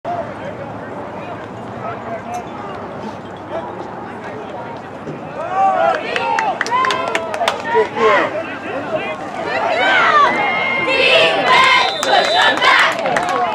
Clovis West vs Clovis North - October 12, 2023 - Freshman Football - MVI 5931
Clovis West vs Clovis North - October 12, 2023 - Freshman Football